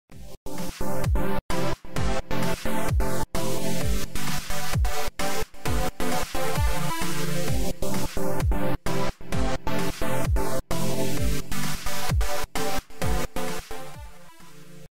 Snow Hill
Snow hill animation in blender, please comment for improvement.